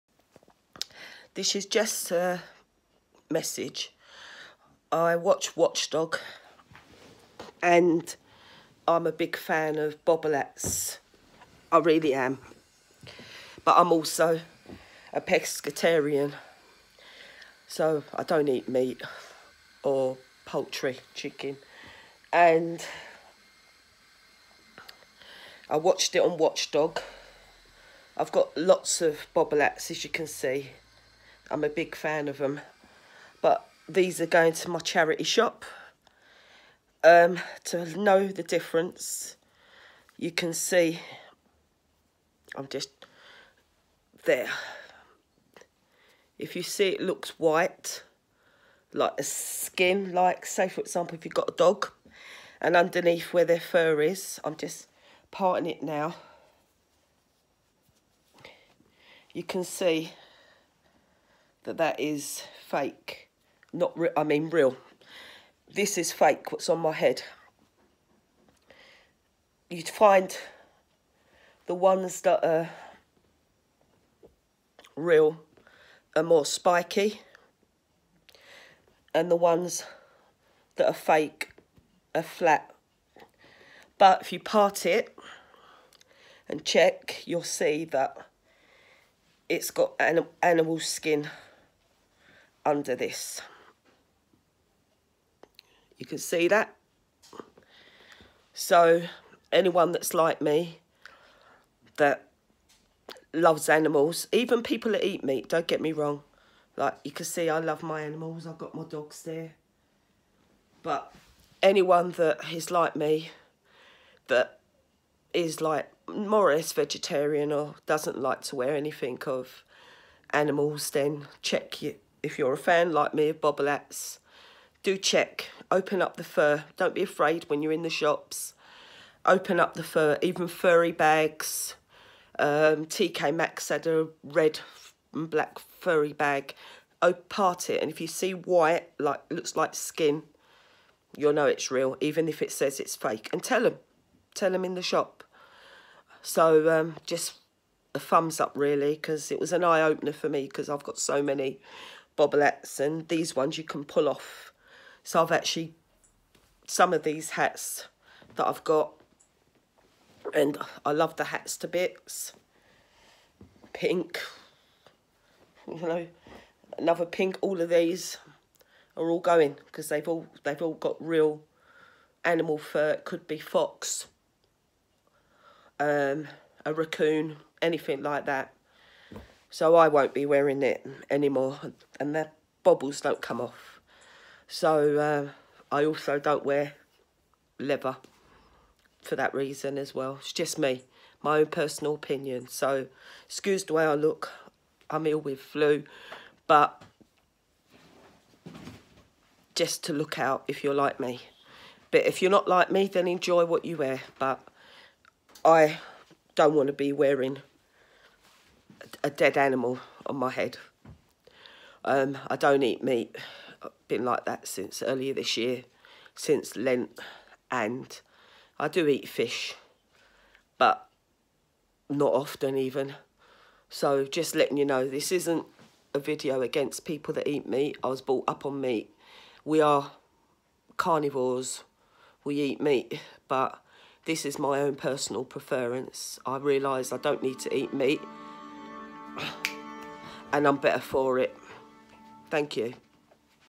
Fake Fur Bobble Hats
After watching bbcwatchdog watchdog about furry bobble hats having real, animal fur, I was so upset that I had many that stated they were fake fur. Unfortunately mine were purchased on a market stall last year so no refund. But I’m so thankful to Watch Dog for the thumbs up for my future buying. Hope this video helps fellow Pescatarians like myself, Vegetarians and Vegans to. Please share and get the word out. Thank you.